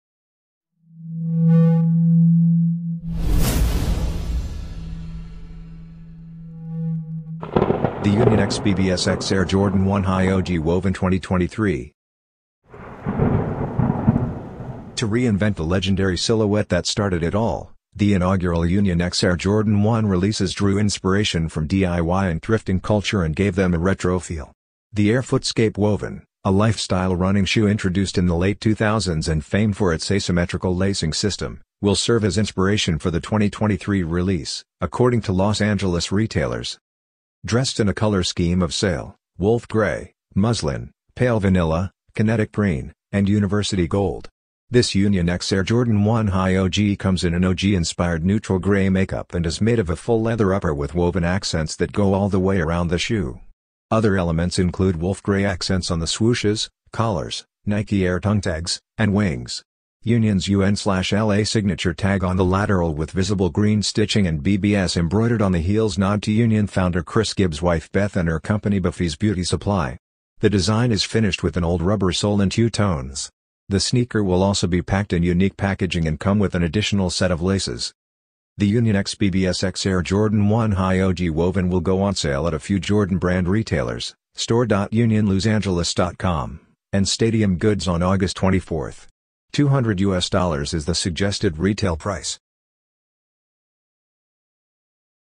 UNION LA x BBS 2023 Air Jordan 1 Retro High OG DETAILED LOOK AND RELEASE INFO
Union LA x BBS
Air Jordan 1 Retro High OG
WOVEN
2023

FOR ADULTS
LIFESTYLE

Colors: Sail - Wolf Grey - Muslin - Pale Vanilla -  Kinetic Green - University Gold
Style Code: FD2565-100

Release Date: August 24, 2023
Retail Price: US$200